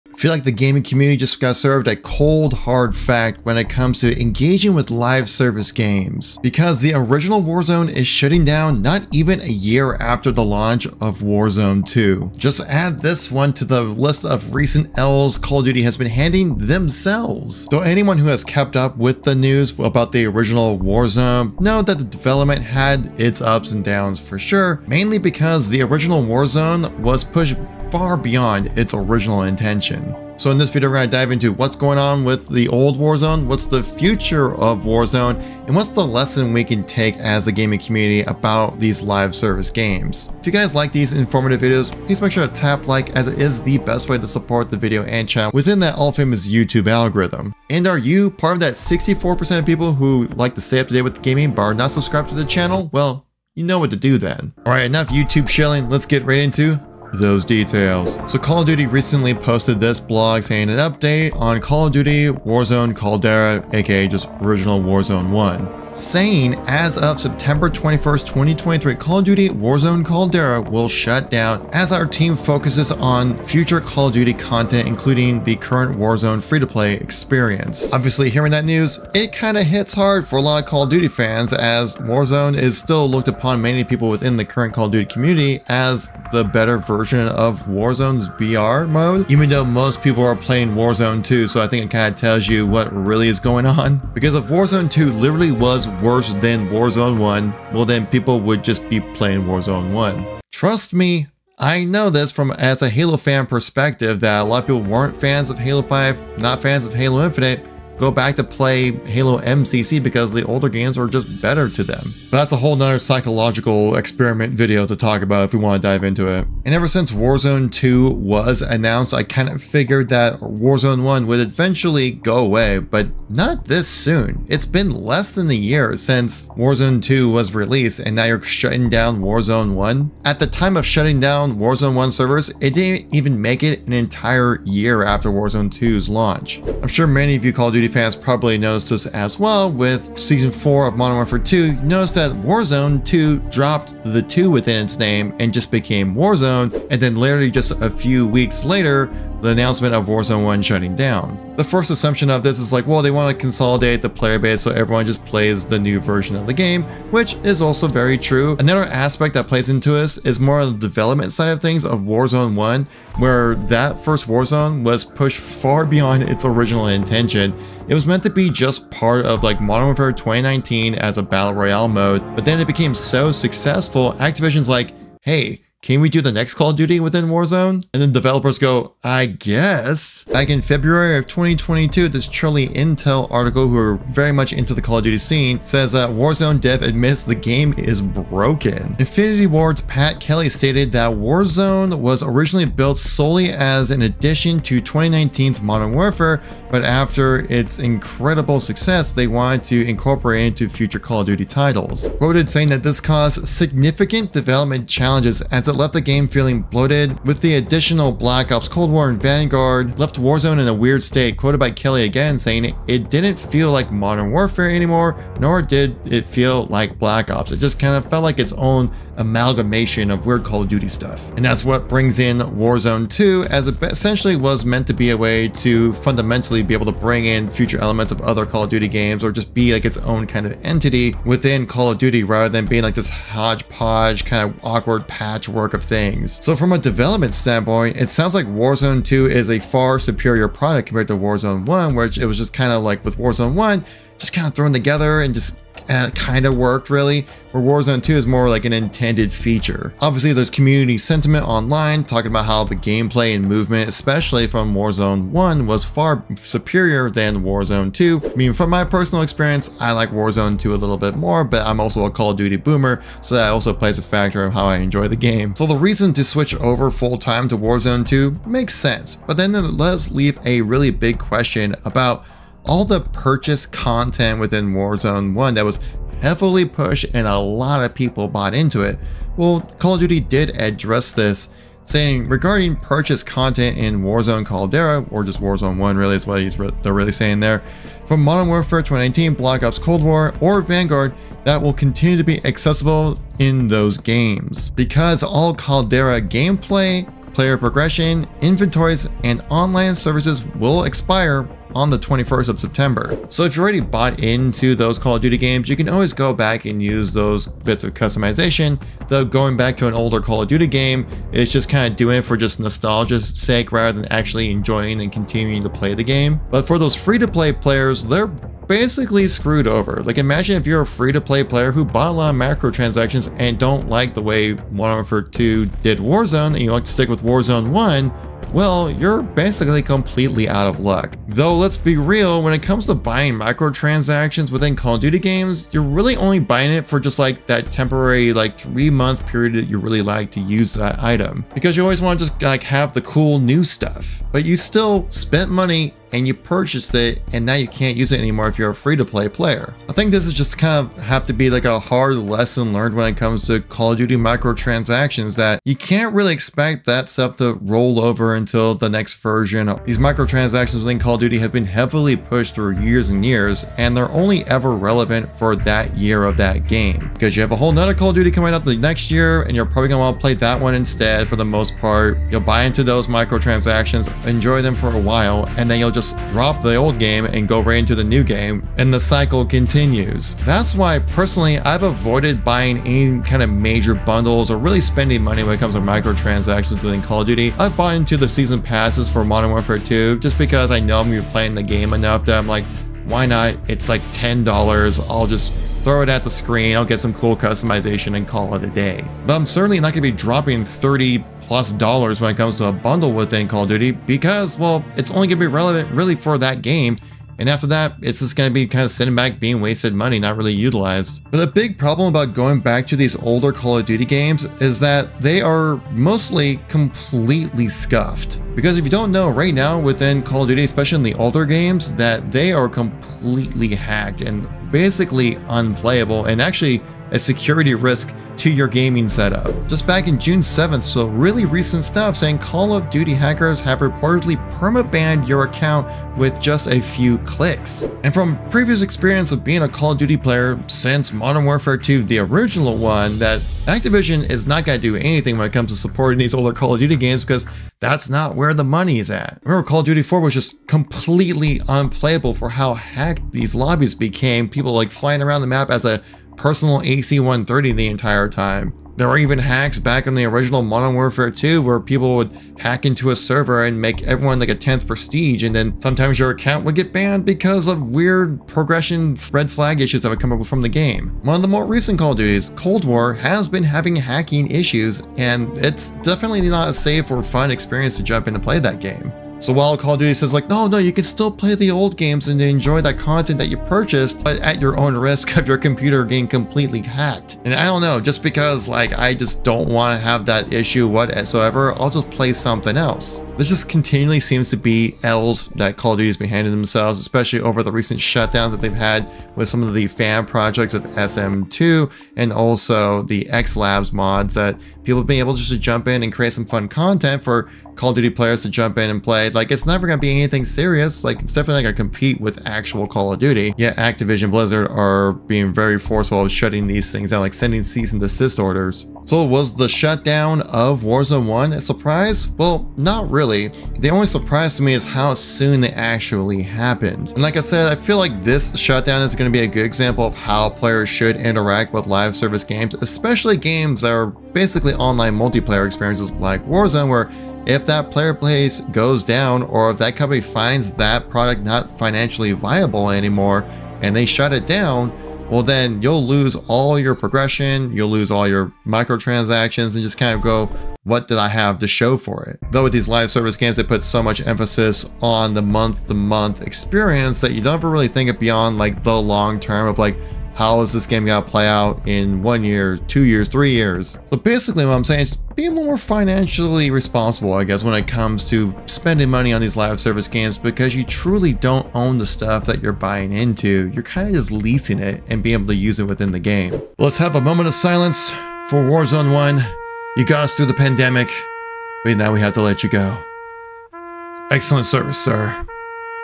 Call of Duty Warzone Servers Shutting Down an Example of Live Service Problem? Call of Duty News
WARZONE 1 Is SHUTTING DOWN! They Are Shutting Down WARZONE RIP Call of Duty Warzone